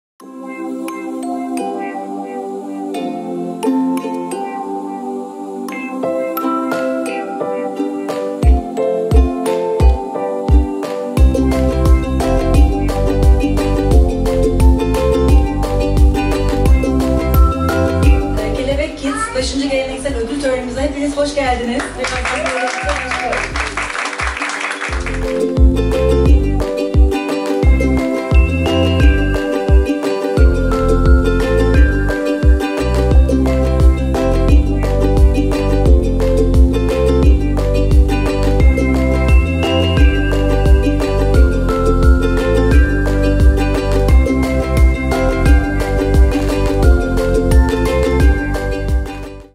5. Geleneksel 23 Nisan Kelebek Kids Resim Yarışması'nın Ödülleri Sahiplerini Buldu!🦋
5. Geleneksel 23 Nisan Kelebek Kids Resim Yarışması'nın Ödülleri Sahiplerini Buldu!🦋

Ödül töreni, sürpriz hediyeler, yüz boyama, kum boyama, animasyon gösterilerinin bulunduğu etkinliğimizden güzel görüntüler sizlerle.🦋